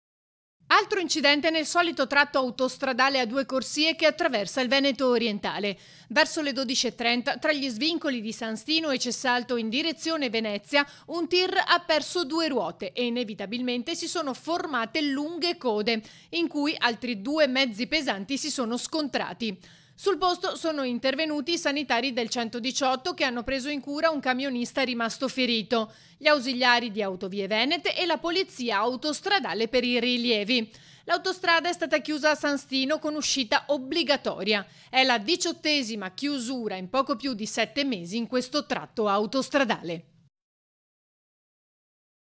TIR PERDE DUE RUOTE, CODE E INCIDENTI NEL TRATTO MALEDETTO | 23/06/2021
A3 NEWS Veneto 23/06/2021 - SANTO STINO DI LIVENZA - Un tir ha perso due ruote, due camion si sono scontrati. E’ successo ancora una volta nel tratto autostradale definito ormai maledetto.   || Altro incidente nel solito tratto autostradale a due corsie che attraversa il Veneto orientale. Verso le 12.30 tra gli svincoli di San Stino e Cessalto in direzione di Venezia un tir ha perso due ruote e inevitabilmente si sono formate delle code in cui altri due mezzi pesanti si sono scontrati. Sul posto sono intervenuti i sanitari del 118 che hanno èreso in cura un camionista ferito, gli Ausiliari di Autovie venete e la Polizia autostradale.  L'autostrada è stata chiusa a San Stino con uscita obbligatoria. È la 18ma chiusura in questo tratto in poco più di 7 mesi. (Servizio di Ilaria Marchiori) - Segui Antenna Tre anche sul digitale terrestre! Visita il sito